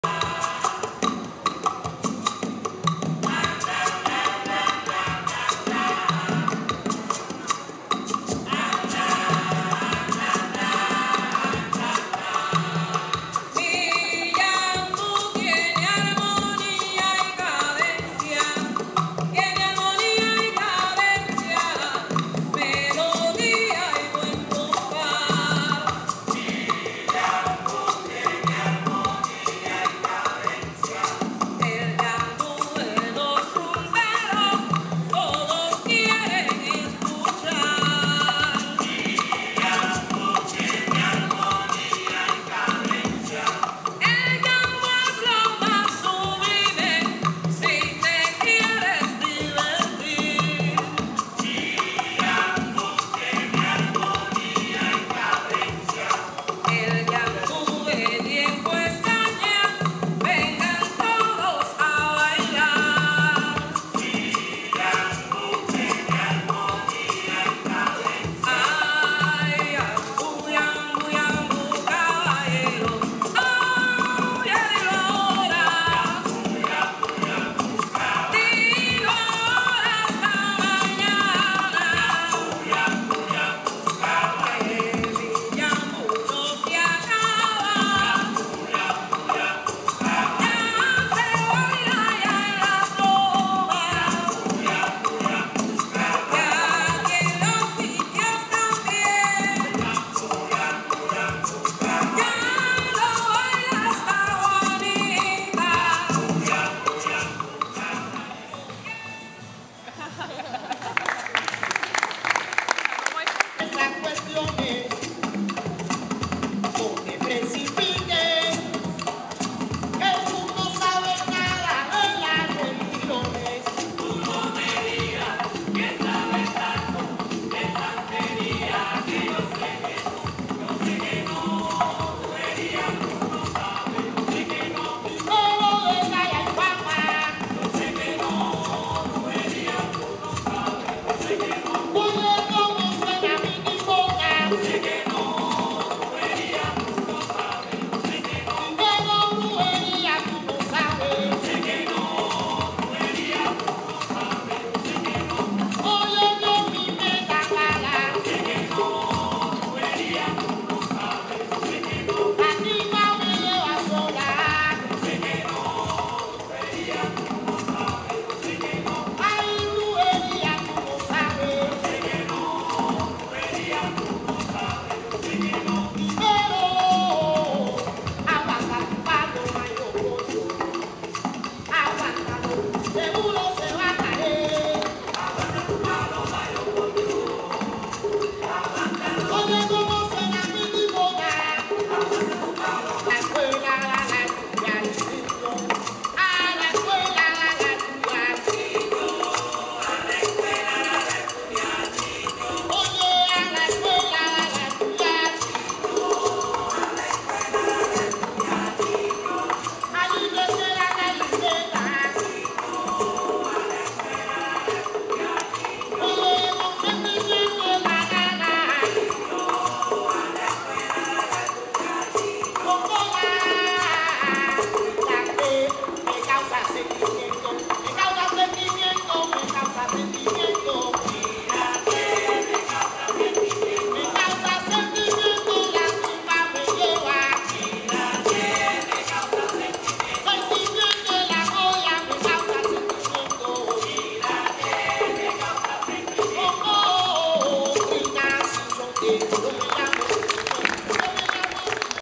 Rumba Demos by Viadanza Team (Cuba)
Club Almendares, Havana - Cuba on Sunday 1 May 2011. Demos of the 3 rumba styles by Viadanza Team: Yambú, Guaguancó and Columbia.